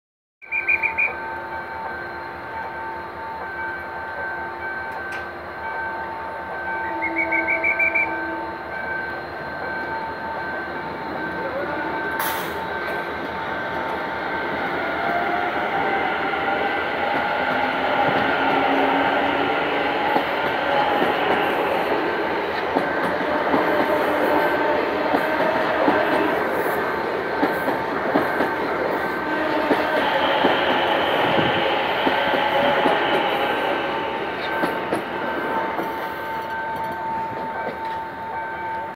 CSR-Mitsubishi RC24 superando en Haedo al tren 152 Bragado-Once con MF105 + 10 coches CNR
El miércoles 3 de agosto de 2022 a las 10.22